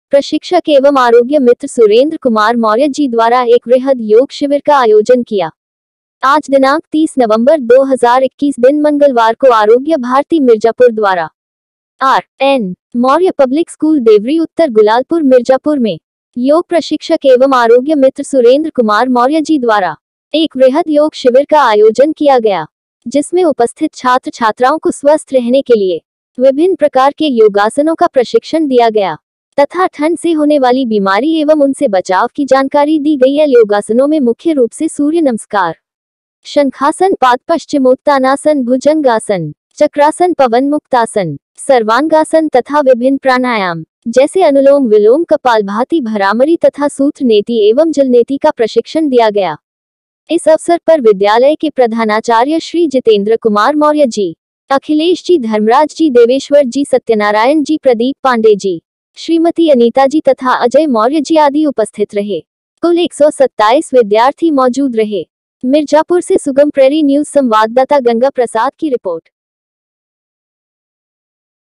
प्रशिक्षक एवं आरोग्य मित्र सुरेंद्र कुमार मौर्य जी द्वारा एक वृहद योग शिविर का आयोजन किया#
आज दिनांक 30 नवंबर 2021 दिन मंगलवार को आरोग्य  भारती मिर्जापुर द्वारा। आर .एन . मौर्य पब्लिक स्कूल देवरी उत्तर गुलालपुर मिर्जापुर में। योग प्रशिक्षक एवं आरोग्य मित्र सुरेंद्र कुमार मौर्य जी द्वारा। एक वृहद योग शिविर का आयोजन किया गया। जिसमें उपस्थित छात्र छात्राओं को स्वस्थ रहने के लिए। विभिन्न प्रकार के योगासनों का प्रशिक्षण दिया गया। तथा ठंड से होने वाली बीमारी एवं उनसे बचाव की जानकारी दी गई l योगासनों में मुख्य रूप से सूर्य नमस्कार। शंखआसन पादपश्चिमोत्तानासन भुजंगासन। चक्रासन पवनमुक्तासन। सर्वांगासन तथा विभिन्न प्राणायाम। जैसे अनुलोम विलोम कपालभाति भ्रामरी तथा सूत्र नेति एवं जलनेति का प्रशिक्षण दिया गया। इस अवसर पर विद्यालय के प्रधानाचार्य श्री जितेंद्र कुमार मौर्य जी। अखिलेश जी धर्मराज जी देवेश्वर जी सत्यनारायण जी प्रदीप पांडे जी। श्रीमती अनीता  जी तथा अजय मौर्य जी आदि उपस्थित रहे। कुल 127 विद्यार्थी मौजूद रहे। मिर्जापुर से सुगम प्रहरी न्यूज़ संवाददाता गंगा प्रसाद की रिपोर्ट